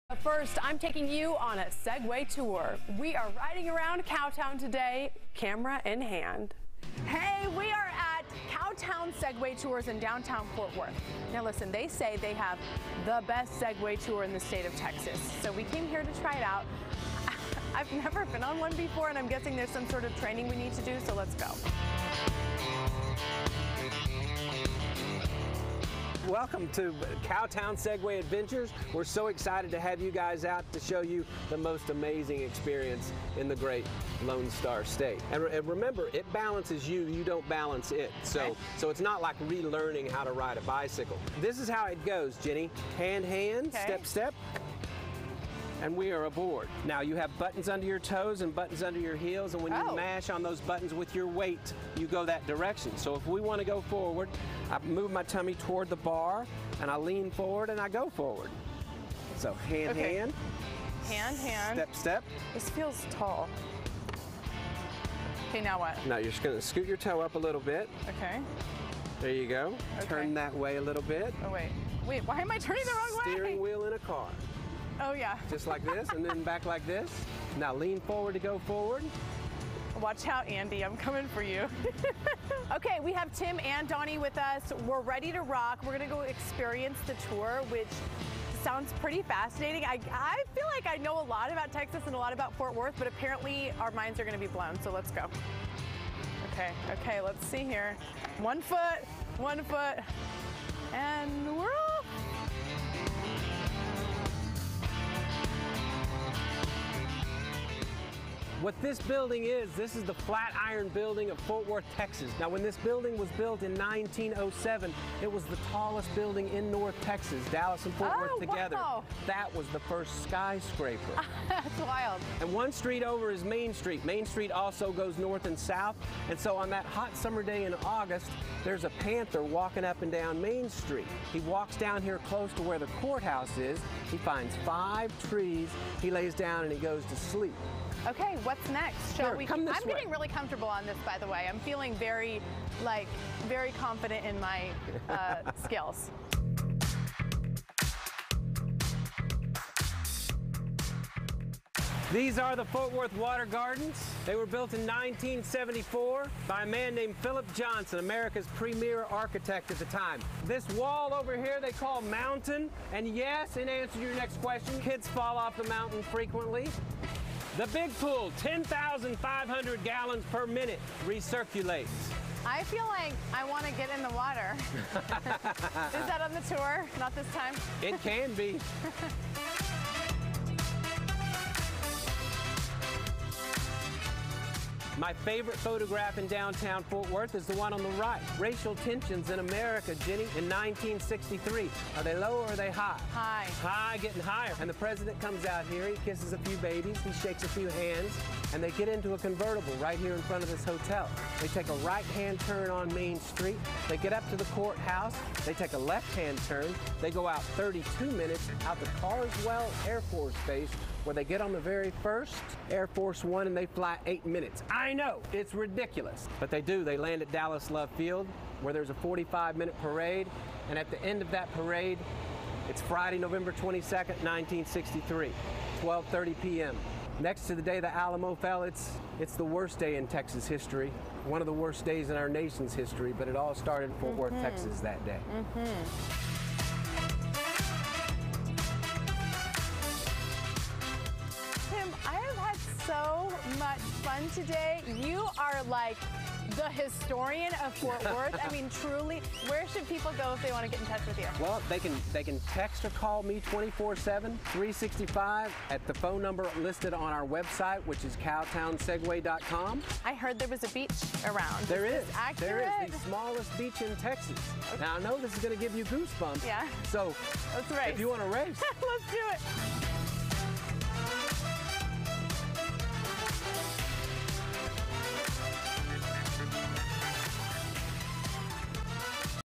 Does Cowtown Segway Tours have the best tours in Texas? We went to find out
Inside DFW